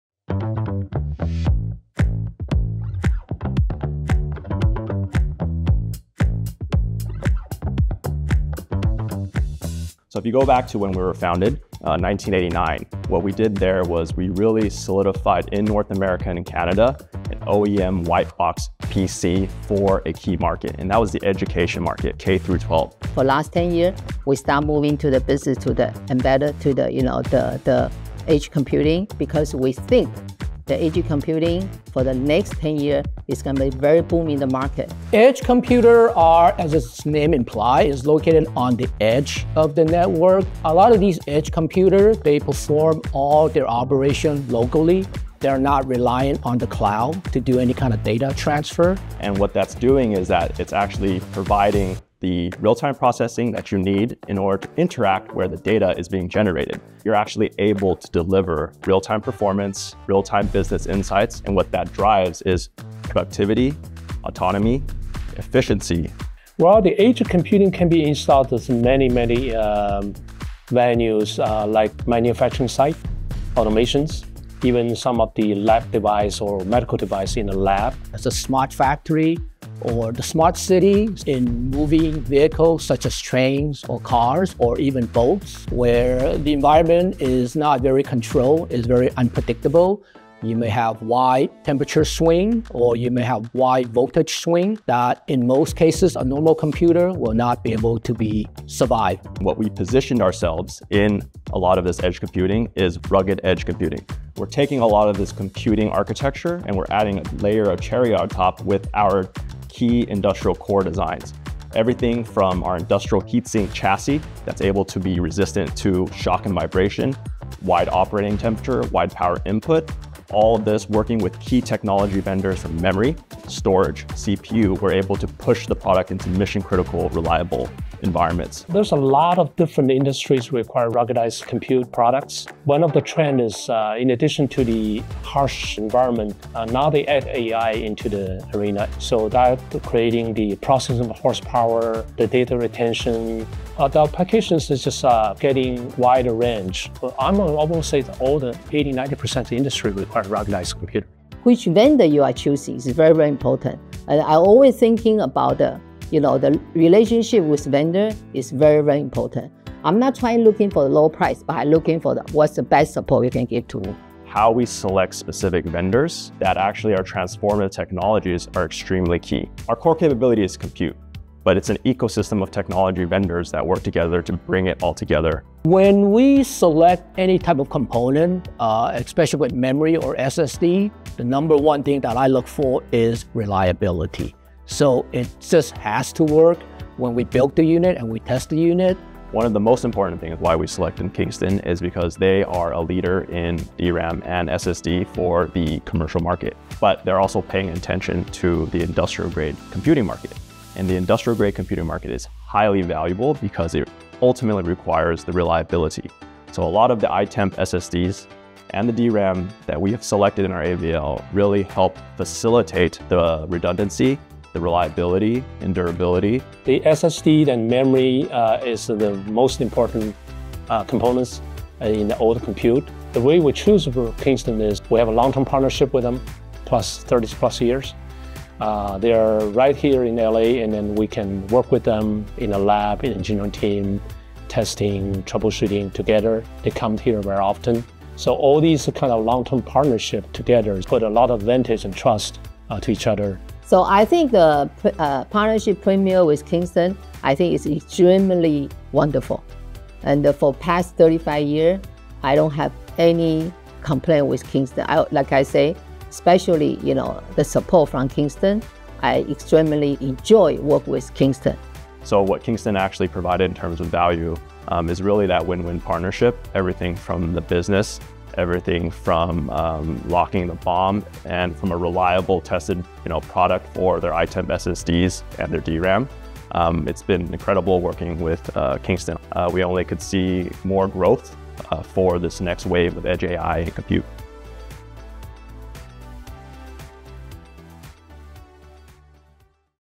Edge Computing with Premio and Kingston: A Case Study
Kingston interviews key figures from edge computing specialist and long-term partner Premio Inc. Pivoting from an initial focus on education, Premio now concentrates on embedded tech and edge computing: devices intended for use in industrial or otherwise physically-demanding environments.
Edge computing is thus called because the computers exist on the edge of the network, not in the data center or server room. They perform most or all of their own tasks, as opposed to shunting data to the cloud. This promotes efficiency, autonomy, and productivity.

However, edge computing requires components that can stand up to unpredictable conditions such as extremes of temperature and variable voltages. With the help of technology vendors such as Kingston, Premio are building rugged edge computing systems able to resist shock, vibration, and high variation in operating temperatures and power input, among other things.

A new factor in the requirements for edge computing is the ability to handle the technological demands of AI. This means more need for processing power.

Premio has carefully considered which partners to work with in order to bring its ruggedized edge computing to the marketplace. Only with a complex ecosystem of vendors can they deliver the products they need. Reliability is one of their primary requirements, and that is why Premio has stuck with Kingston for decades. Kingston’s industry-leading reputation and increased focus on industrial computing makes us highly suitable partners for Premio. We prioritize service and quality, to deliver components that make the future of industry possible.